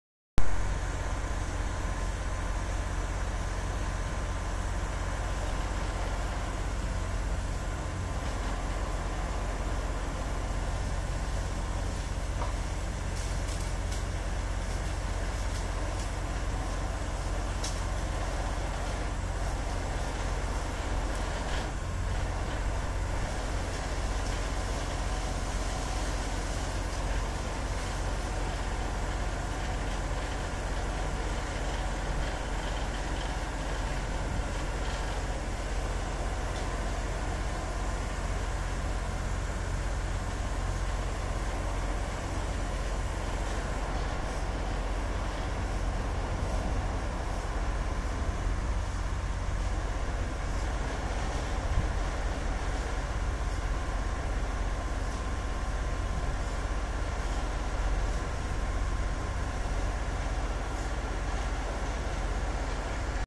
Raccolta ramaglie - manutenzione del verde
Manutenzione condominiale, raccolta ramaglie dopo potatura invernale.